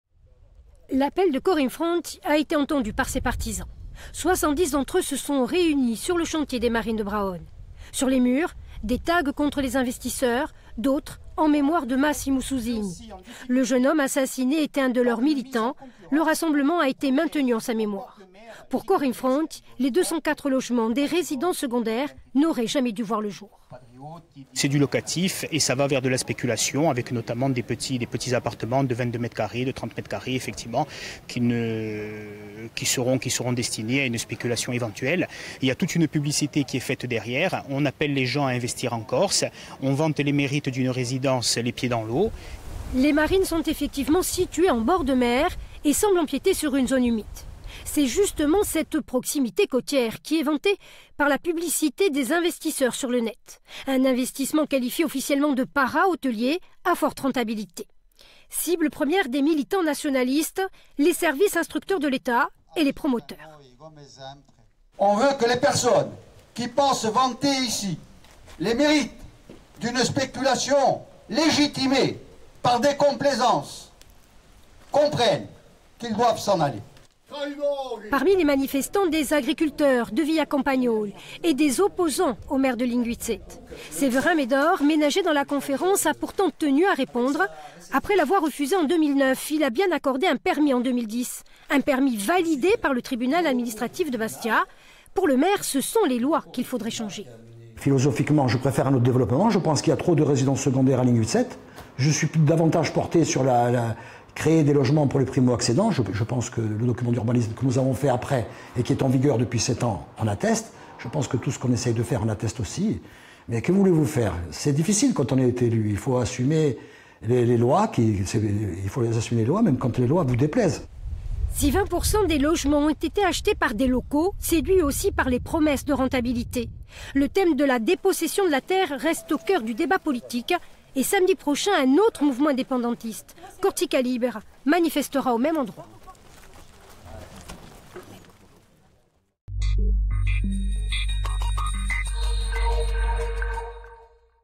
Linguizzetta : Core in Fronte mobilisé contre le projet immobilier « Marina di Bravone »
Core in fronte a organisé un rassemblement ce samedi 14 septembre à Linguizzetta. Le mouvement nationaliste dénonce la construction de 204 logements près du littoral. Un programme que ces militants rejettent au nom de la lutte contre la spéculation immobilière.

Intervenants - Baptiste Lucciardi, Porte-parole " Core in Fronte " ; Paul-Félix Benedetti,Chef de file de " Core in Fronte " ; Severin Medori, Maire de Linguizzetta.
Equipe - Anne-Marie Leccia ; François-Albert Bernardi ; Christophe Gineste.